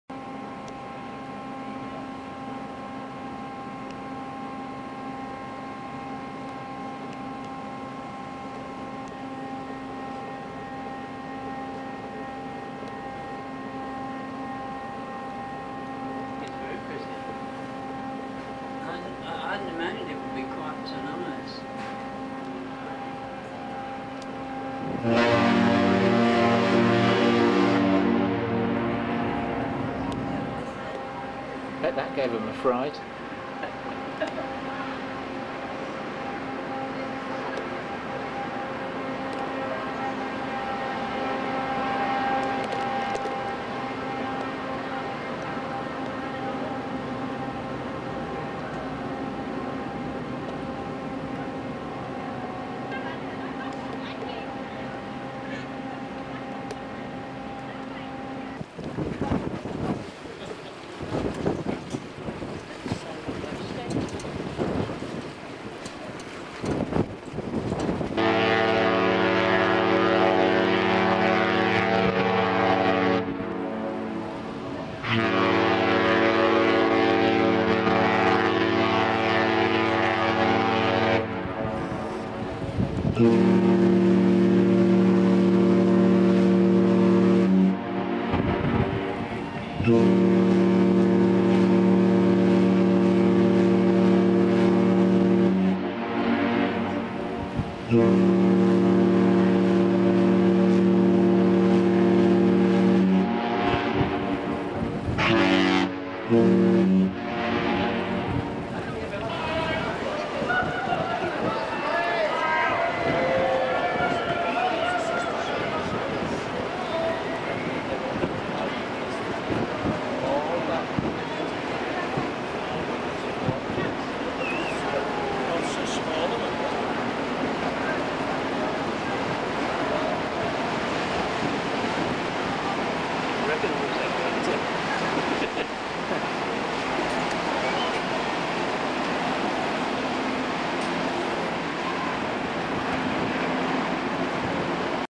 Kiel Canal - Black Watch & Royal Viking Sun
Aboard "Black Watch" in 1999, passing "Royal Viking Sun" and exchanging ships' whistles.(The first 2blasts are from Black Watch; the response is from RV Sun)